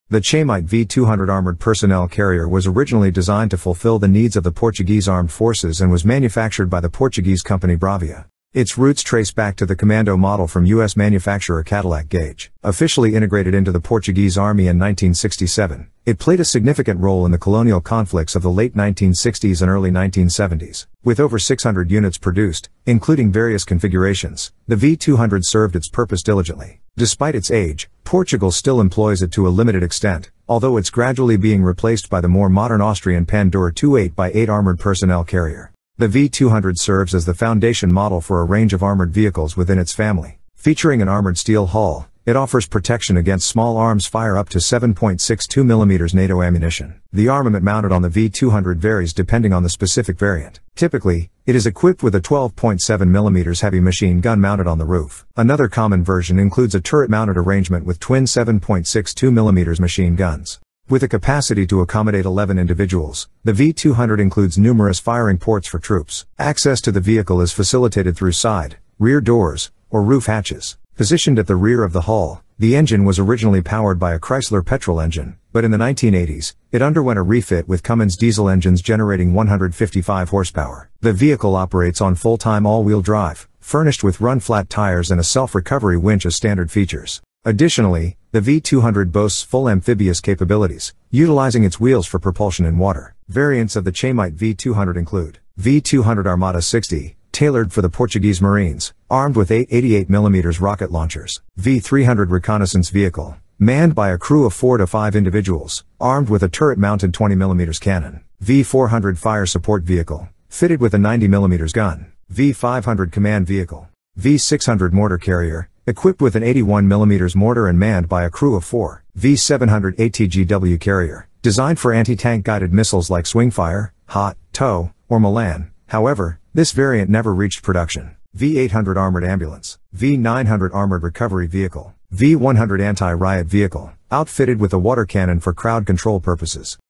Chaimite V 200 Armored personnel carrier
Portugal continues to maintain a limited fleet of the Bravia Chaimite V-200 Armored Personnel Carriers (APCs).